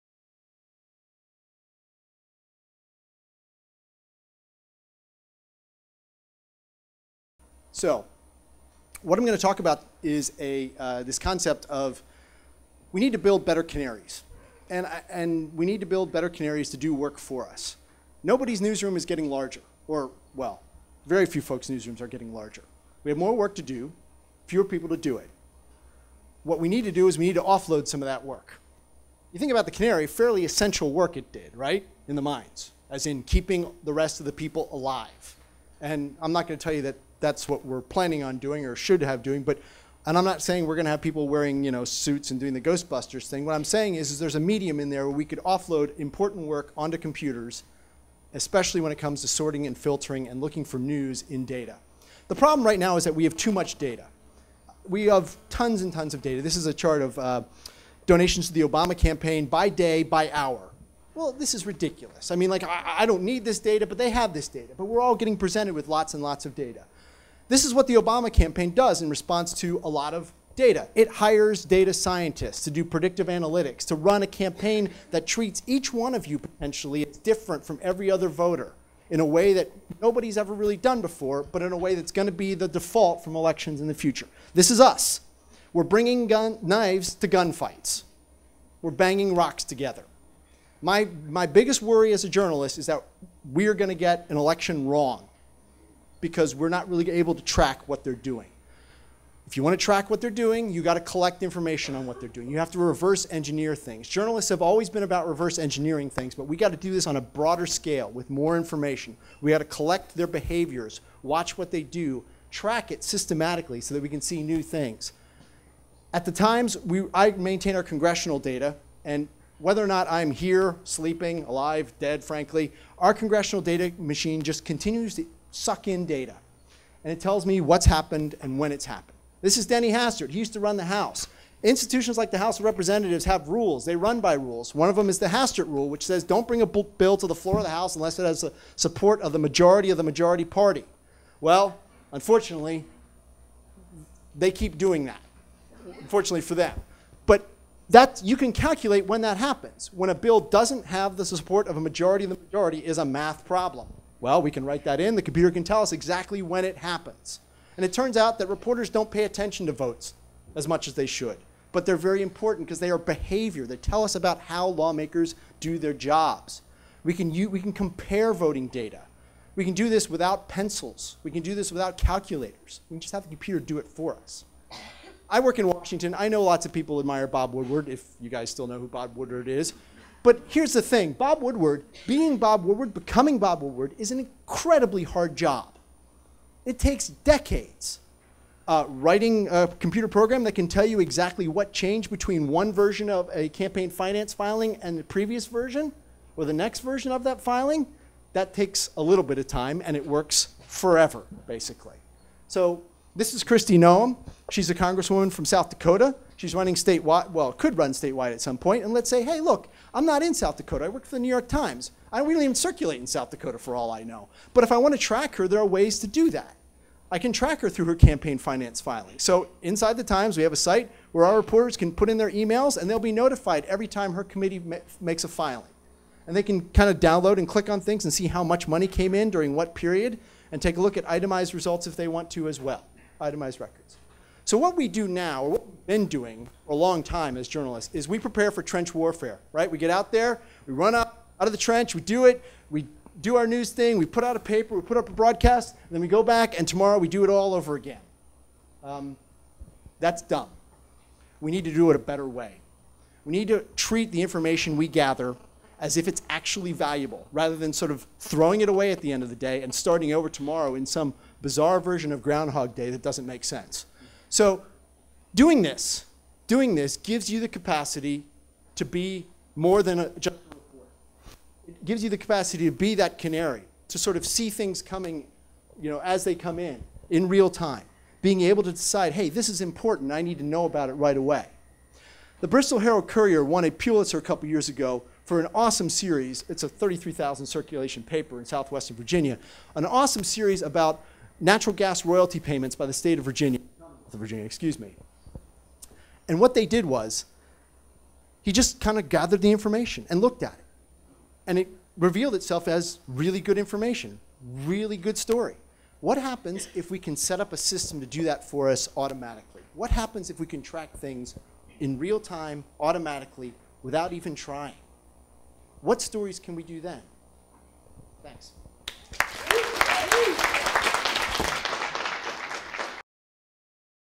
Derek Willis: "Coding for the Future" presentation
Derek Willis, an interactive developer with The New York Times, was a panelist with the WVU School of Journalism's "Coding for the Future: The Rise of Hacker Journalism" event on April 15, 2013. The event was part of the School's new "Future of Media - NOW!" speaker series, which will explore current trends and emerging practices in media and journalism. "Coding for the Future" brought to campus tech-savvy journalists who are engineering change on the front lines of big data. Spinning code into content, this new breed of "hacker journalist" is helping to transform digital storytelling for modern audiences -- ushering in a new relationship between audience and data, code and content, and the past and future of media. Panelists kicked off the event with Ignite-style presentations.